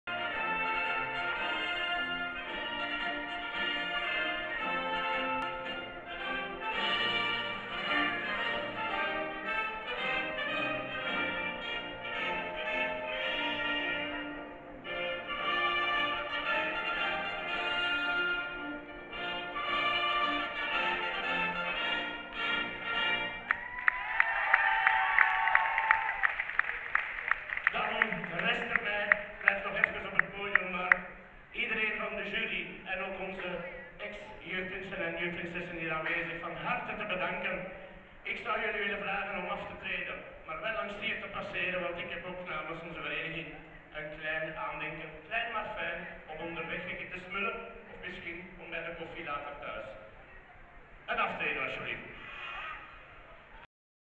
2022-11-13 Jeugd van Belgium aftreden en aanstelling in Zellik-Asse
een zeer denderende namiddag waar veel belangstelling voor was, zeker na al onze Corona tijdperk toen alles stil lag, dus nu Carnaval is in zijn startblokken en vertrokken. Her en der zijn nu overal nieuwe Prinsen & Prinsessen, Alaaf, Alaaf, Alaaf